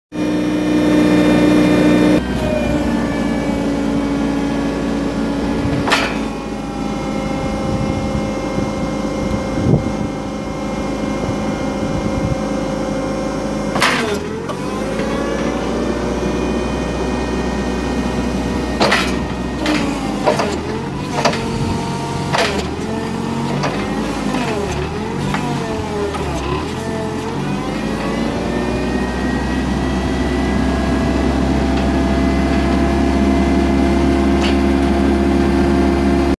Mitsubishi FG25 forklift for sale at auction | bidding closes October 9, 2018
Mitsubishi FG25 forklift
2,326 hours on meter
1F-1R gears
Canopy
Work lights
5,000 lbs max lift capacity
130" max lift height
Three stage mast
Side shift
Mast tilt
40" fork length
7.00-12 front tires
6.90-9 rear tires
Unit starts, but will not move
Equip# HP02
Serial F17A-54341A